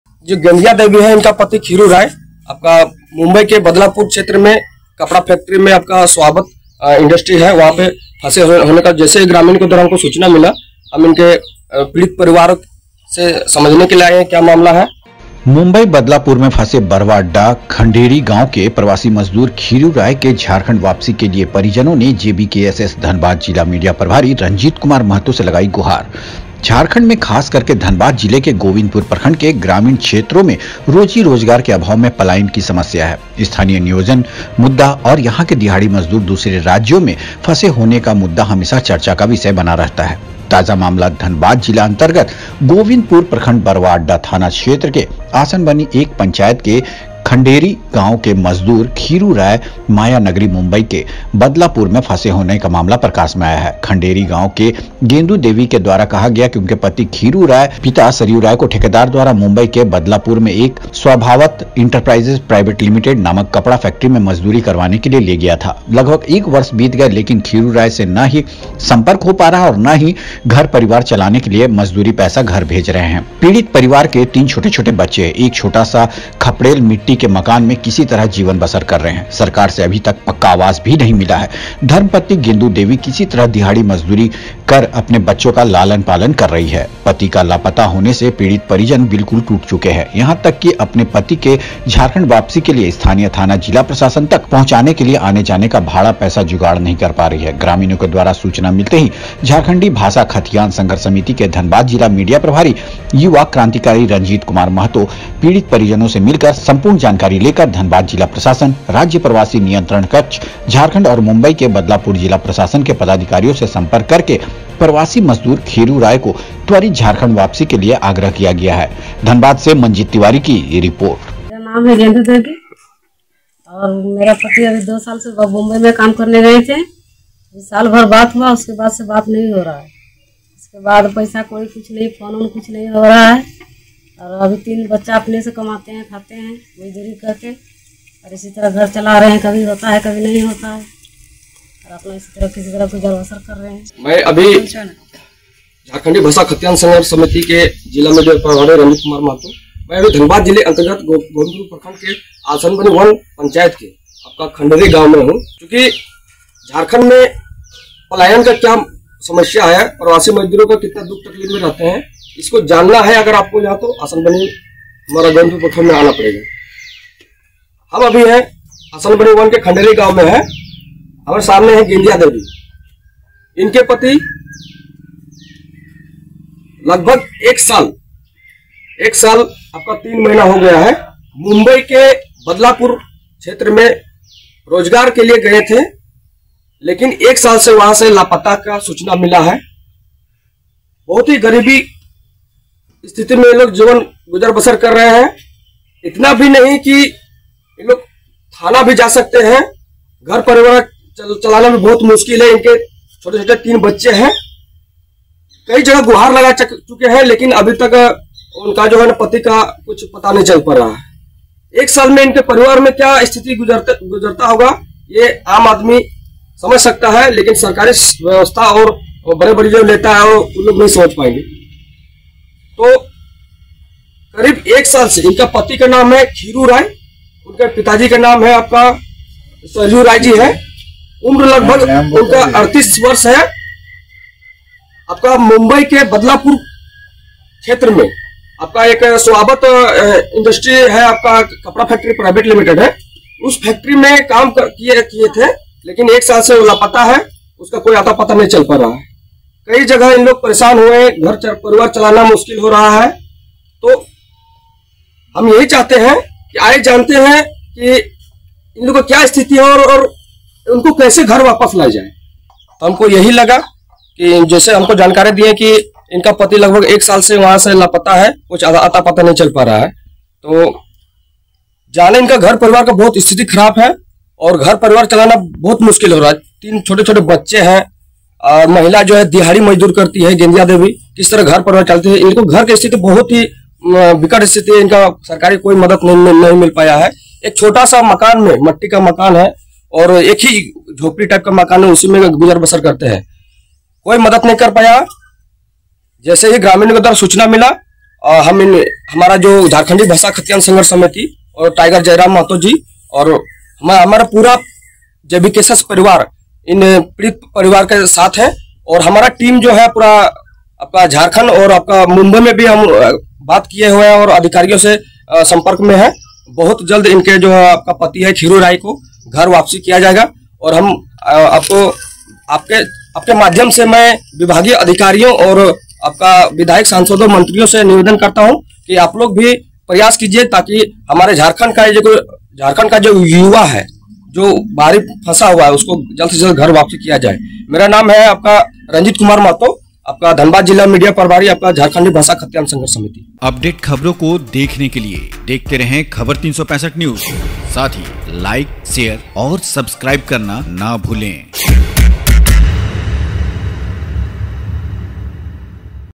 मुंबई में फंसे बरवाअड्डा खंढ़ेरी गांव के प्रवासी मजदूर ख़िरु राय को लाने के लिए jbkss हुई आगे
Awareness first.